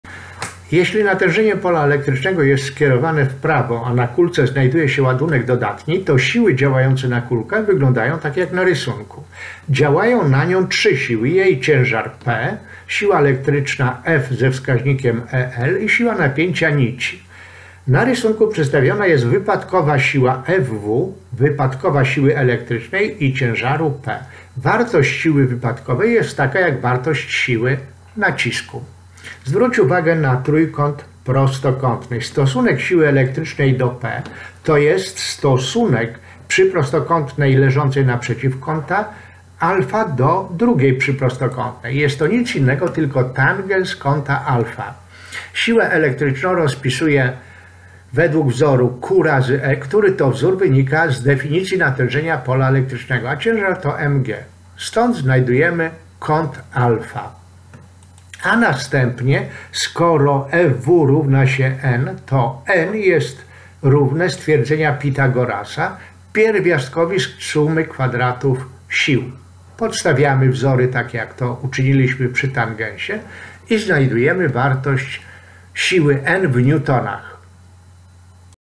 511. Fizykamax, elektrostatyka, wahadło w jednorodnym polu elektrycznym, natężenie pola
Wahadło w jednorodnym polu odchyla się od pionu. Ciężar, napięcie nici. Siła elektryczna. Ładunek. Natężenie pola elektrycznego. Przewodnik elektryczności